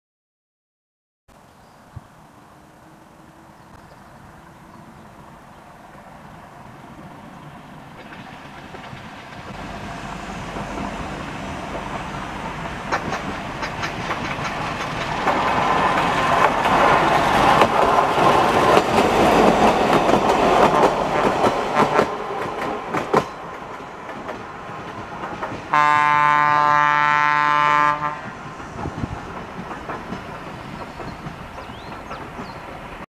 Buscarril Talca Constitución - Sector Unihue
El tren del Maule, a su paso por el sector de Unihue, comuna de Maule , en dirección Constitución.